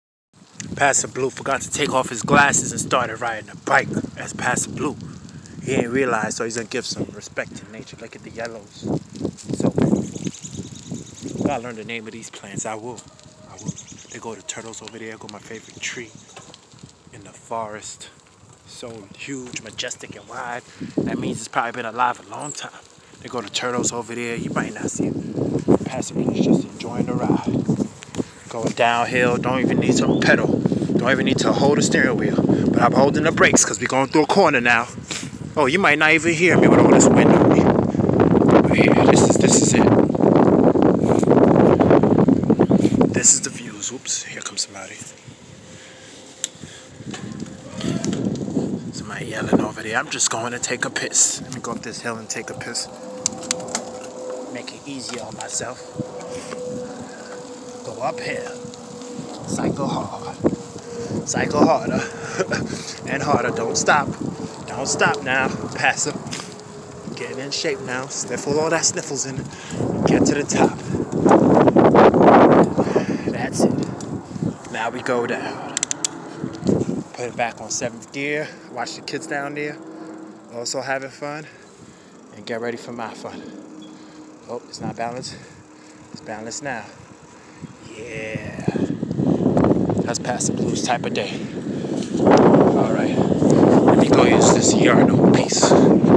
Passa Blue ǀ Rides His Bicycle
Passing through Prospect Park absorbing nature while nature calls me.